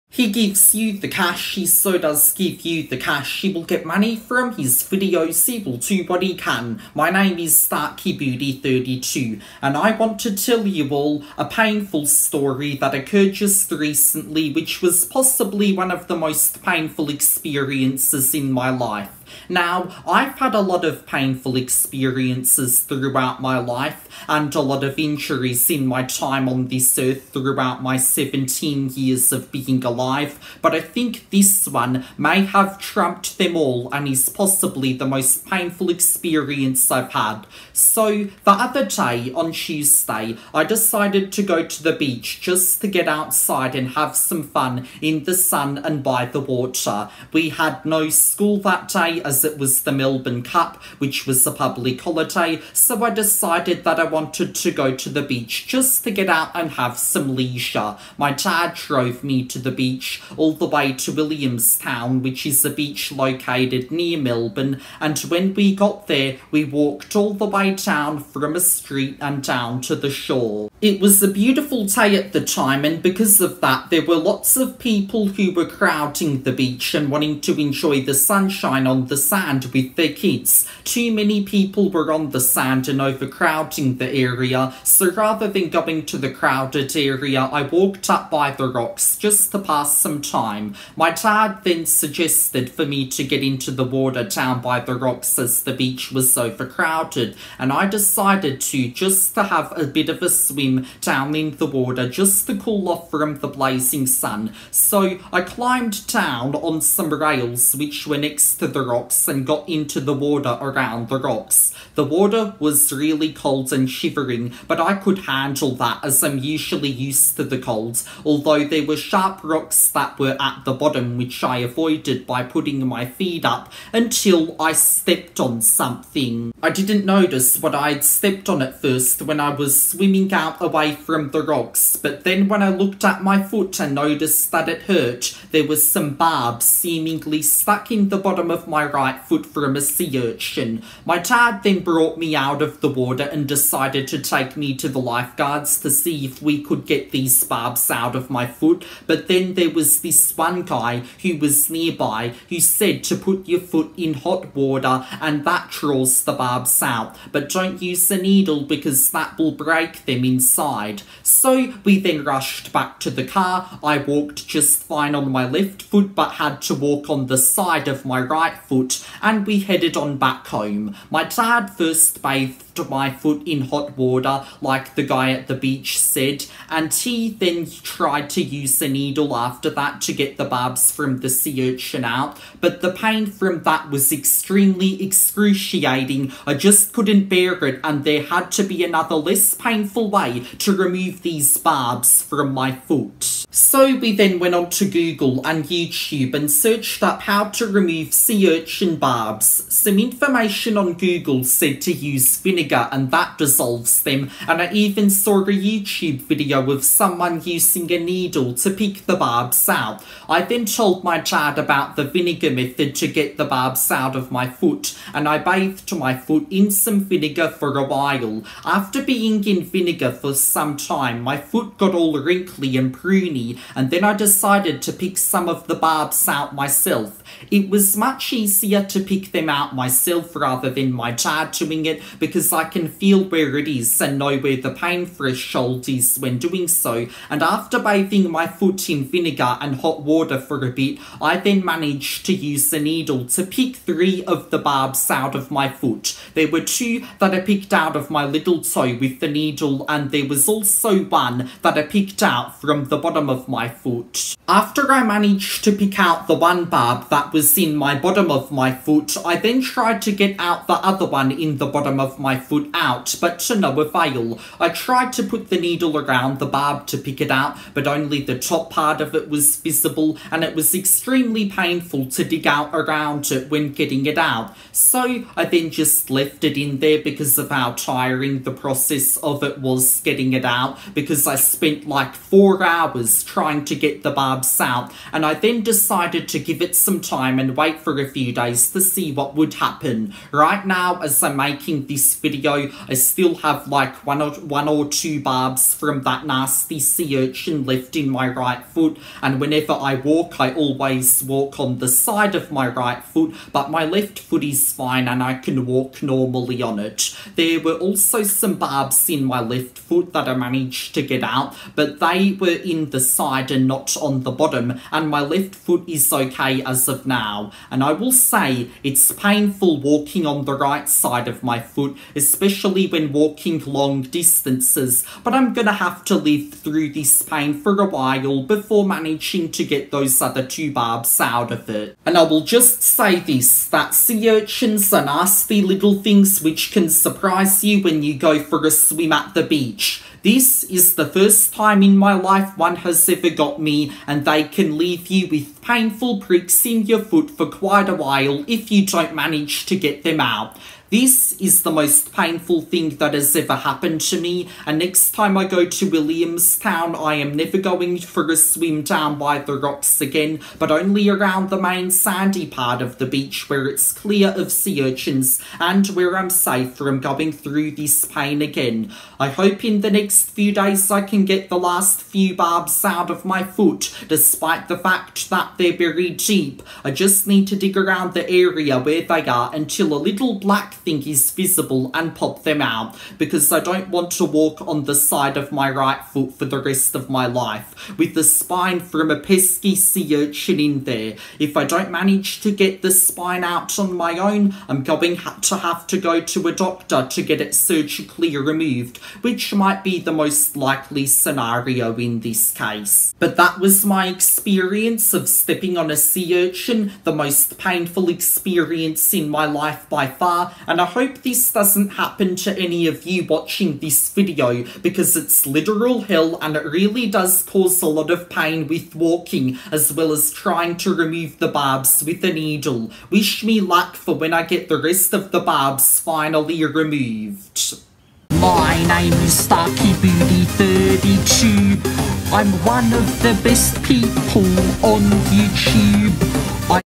I Stepped On A Sea Urchin!
I had the most painful experience of my life the other day by stepping on a sea urchin when I went to the beach, I decided to share that experience in this video to tell you how painful it was.